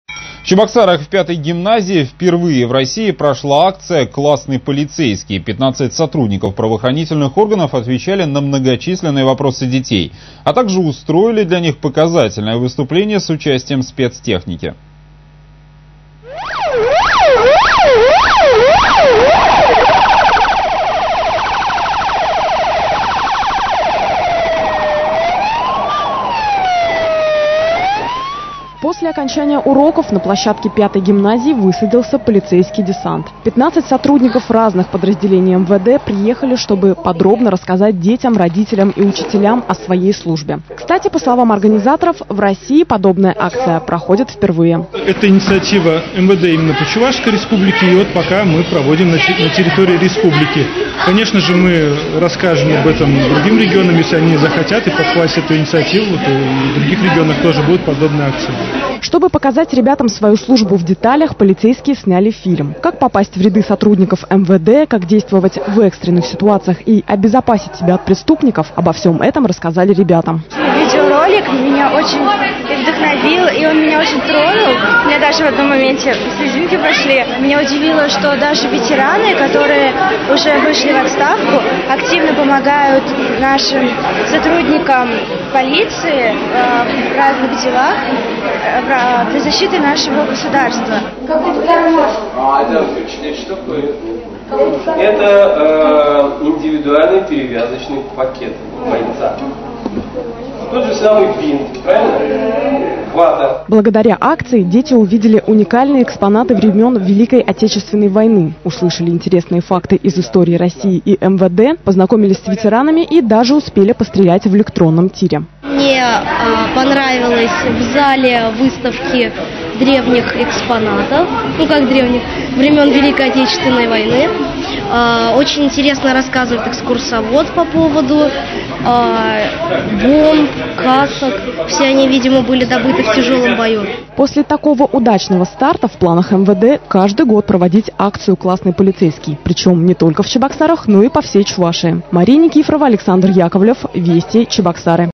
Классный полицейский
Сотрудники МВД рассказали чебоксарским гимназистам о своей работе